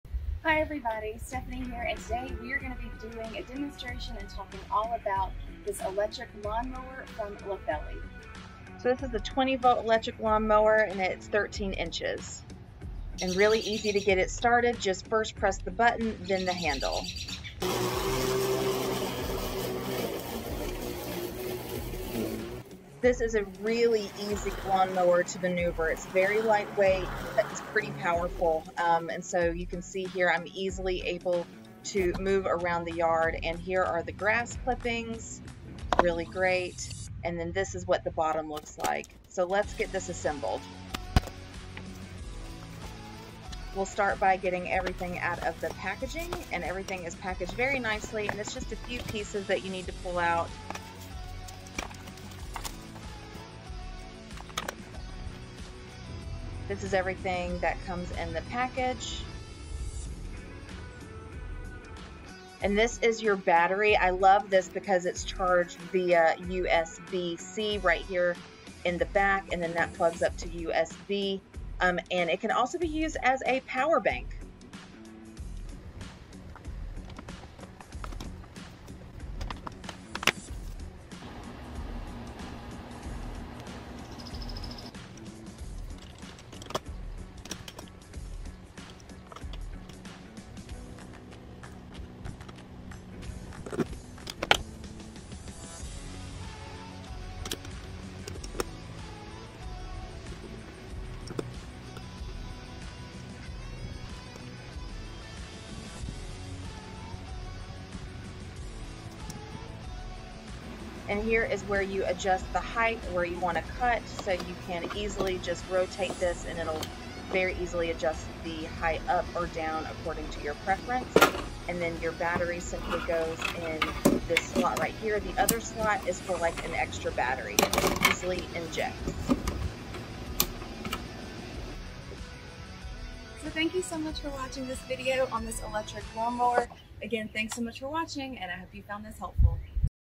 I Tested the Litheli Electric Mower and the Results Were Mind-Blowing!
🌿 Welcome, lawn enthusiasts! Join me, Stephanie, as we delve into the realm of lawn care with the Litheli Cordless Lawn Mower. 🌱 In today's video, we'll embark on a comprehensive demonstration and discussion surrounding this remarkable electric lawn mower.

👉 Ready to revolutionize your lawn care routine? Click the link below to discover the unparalleled performance and convenience of the Litheli Cordless Lawn Mower! 🌿 Let's elevate your lawn game together!

Crafted with precision and innovation, this 20-volt electric lawn mower stands at a modest 13 inches, boasting both efficiency and ease of use. 🌟 With just a simple press of a button followed by the grasp of its handle, this mower springs to life effortlessly, promising a hassle-free mowing experience.

One of the most striking features of this mower is its exceptional maneuverability. 🏃‍♂️ Despite its lightweight design, it packs a powerful punch, effortlessly gliding across your lawn with grace and precision. Witness as I navigate through the verdant expanse of my yard, effortlessly tackling unruly grass clippings with finesse.

Now, let's delve into the assembly process. 🛠️ With meticulous packaging and just a handful of components to assemble, getting started with this mower is a breeze. Unveil the contents of the package to reveal its sleek components, including the convenient USB-C rechargeable battery, which doubles as a power bank for added versatility.

Adjusting the cutting height is a cinch with the intuitive height adjustment mechanism, allowing you to tailor your mowing experience to your exact specifications. Simply rotate the knob to effortlessly raise or lower the cutting height to your desired level.

Bid farewell to the hassle of tangled cords and cumbersome gasoline engines. 🚫 Embrace the future of lawn care with the Litheli Cordless Lawn Mower, where convenience meets performance.

Thank you immensely for tuning in to our demonstration of this electric marvel. Your support means the world to us!

Until next time, happy mowing!